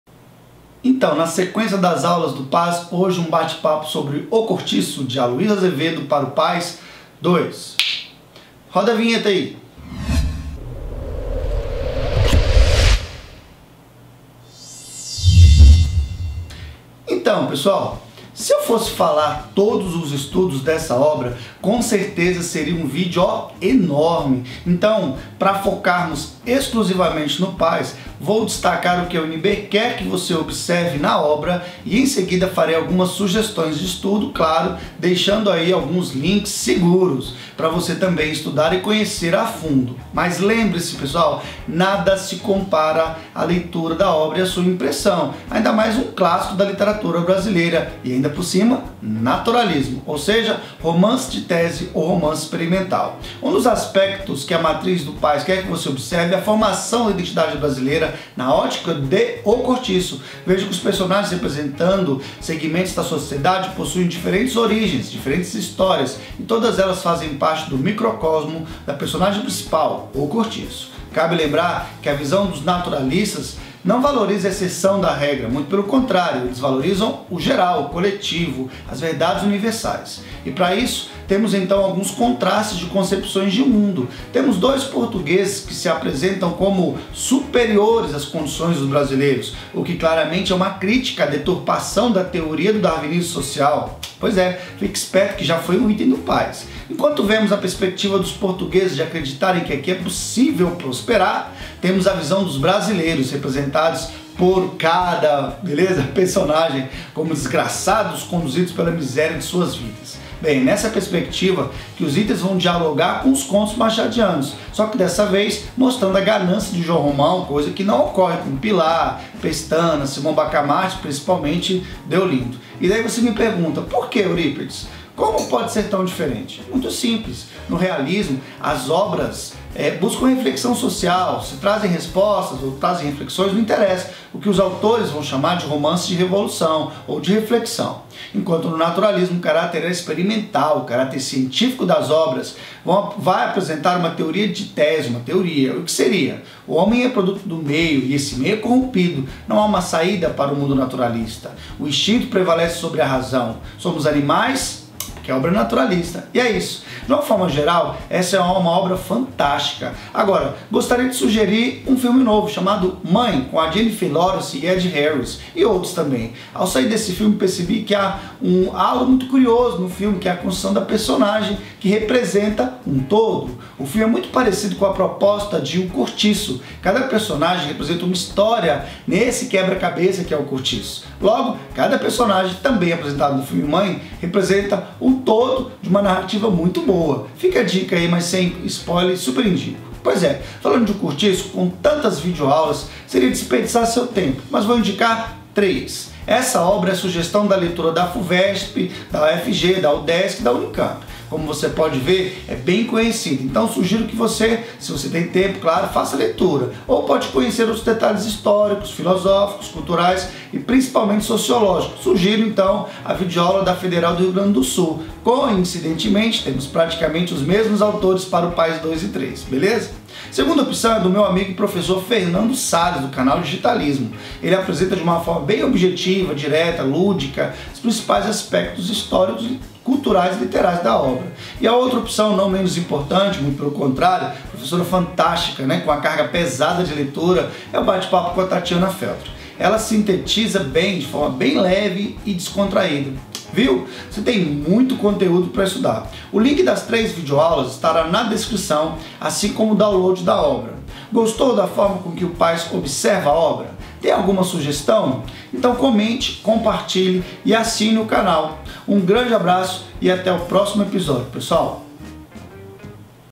OBRA ANTIGA 2018 - PAS 2 - O Cortiço - análise e comentários.
O Cortiço é uma das grandes obras da literatura brasileira e o PAS tem uma nova proposta de leitura e diálogo com outras obras. Venha e confira.